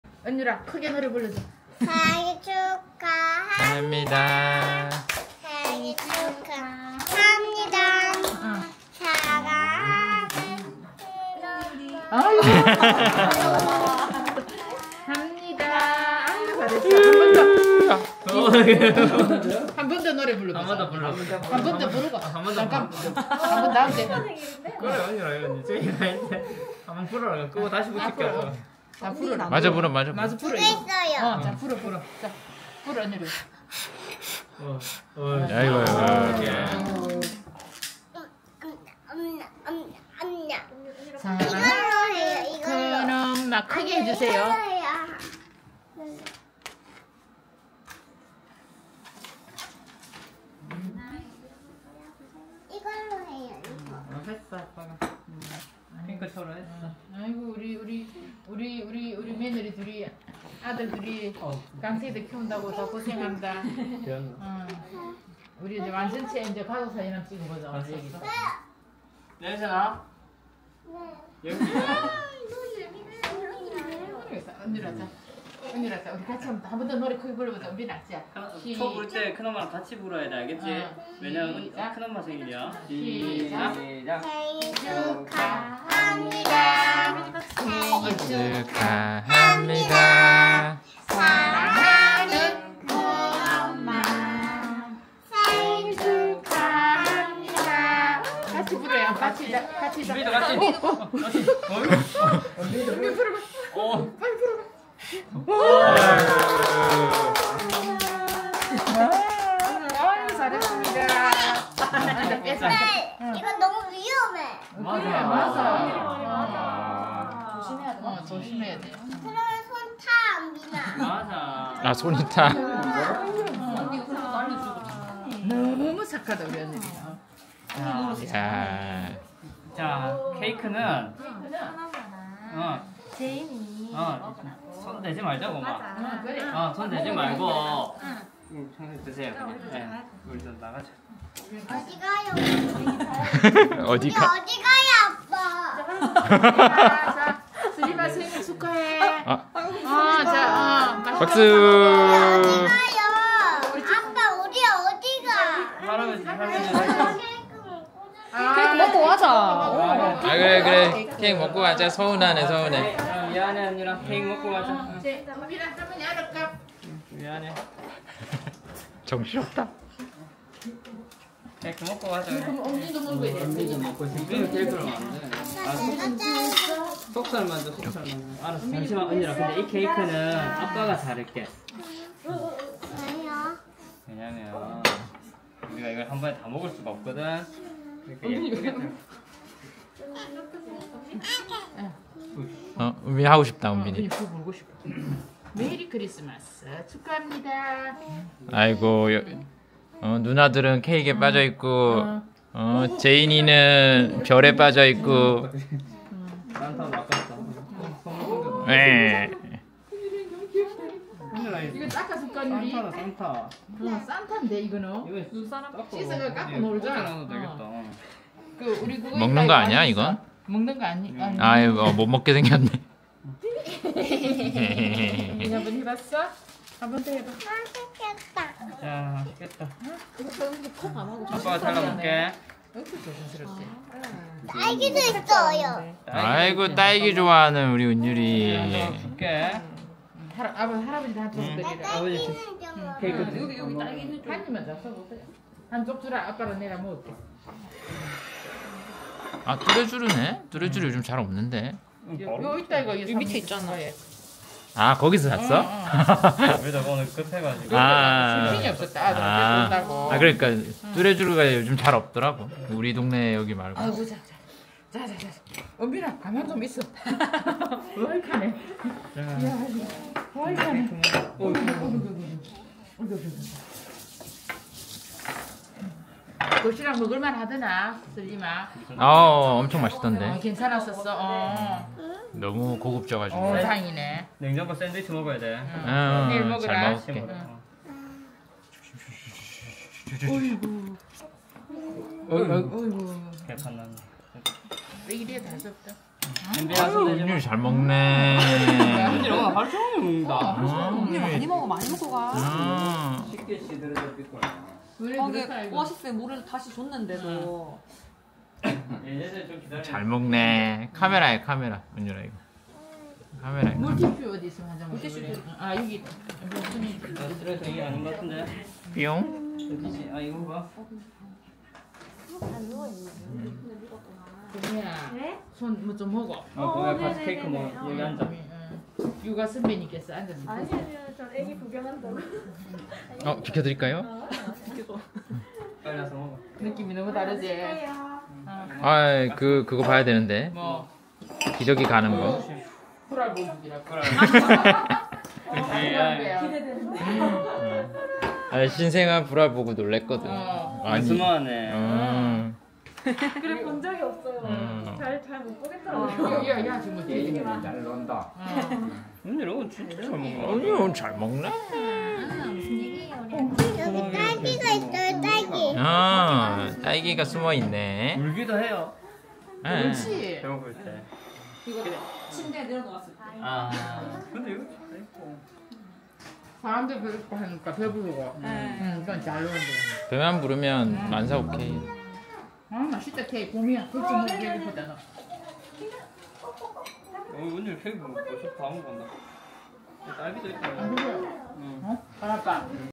2023.12.23. 재인이 53일차 리미 생일
1주일 지난, 늦은 와이프 생일 파티, 고생이 많은 울 아내